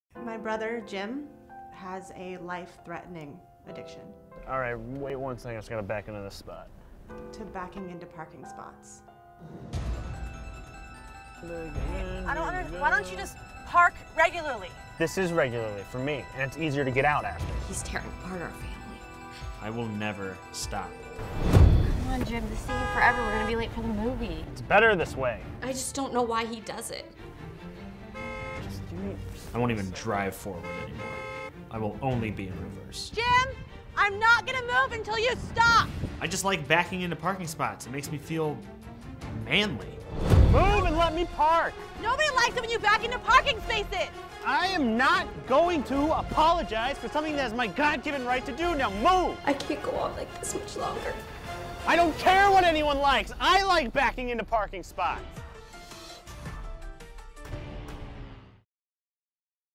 INTERVENTION PREVIEW: Backing Into Parking Spaces
It's tearing this family apart.

Written by: Richelle Meiss
Edited by: Andrew Flack

Starring:
Jeff Muerer
Richelle Meiss
Malie Mason